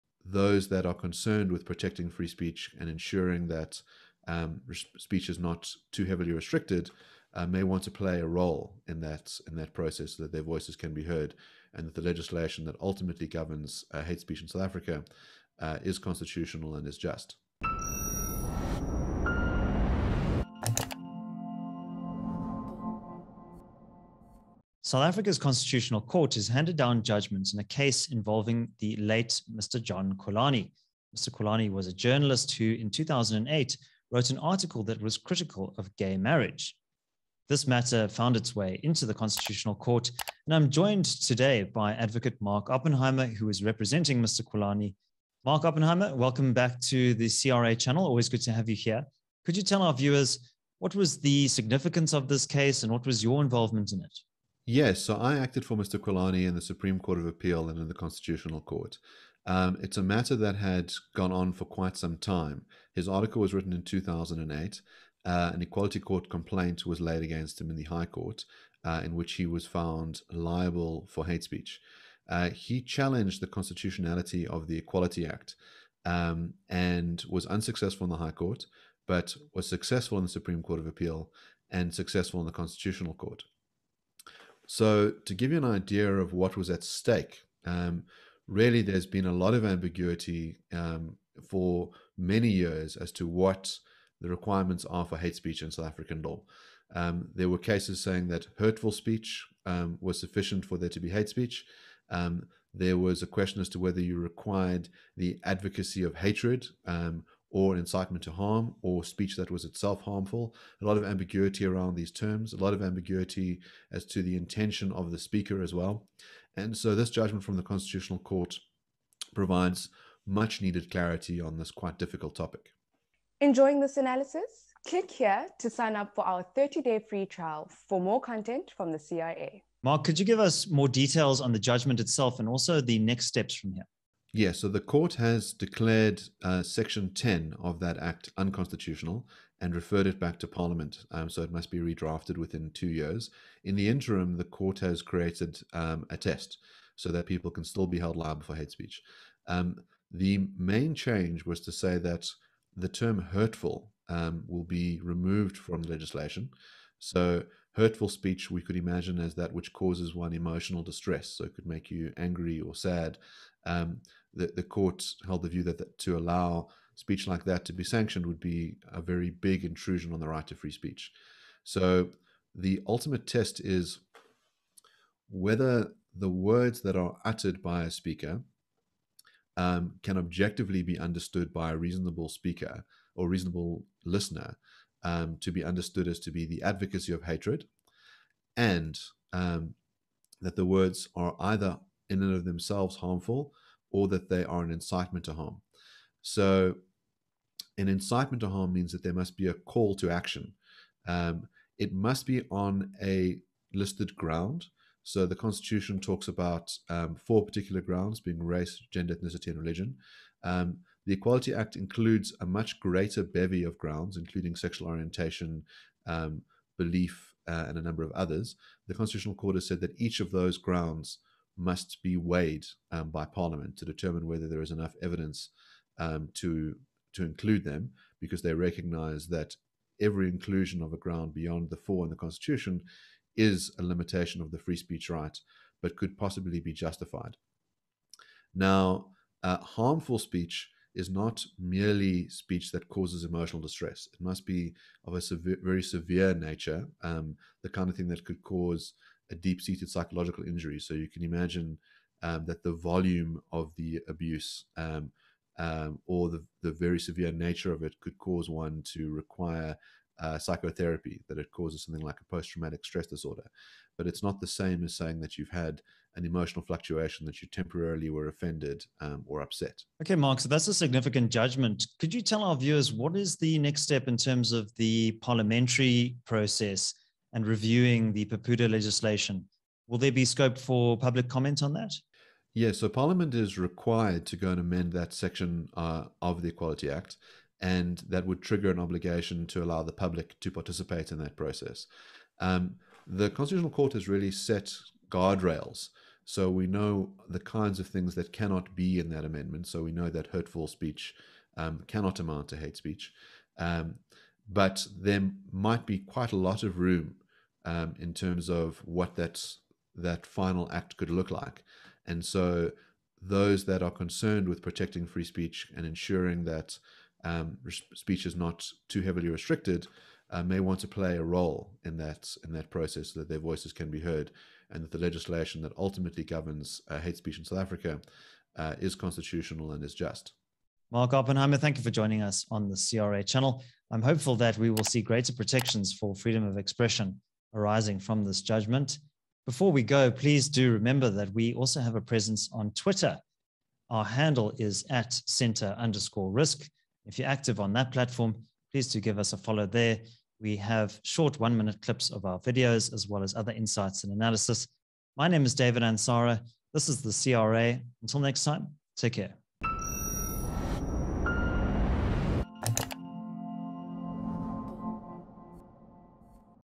A LANDMARK ruling for free speech | Mark Oppenheimer
The Constitutional Court has handed down a landmark judgement which will have significant implications for the protection of free speech. David Ansara of the CRA speaks to Adv. Mark Oppenheimer of the Johannesburg Bar about the details of the case, as well as the amendments that will now be required to South Africa's hate speech legislation.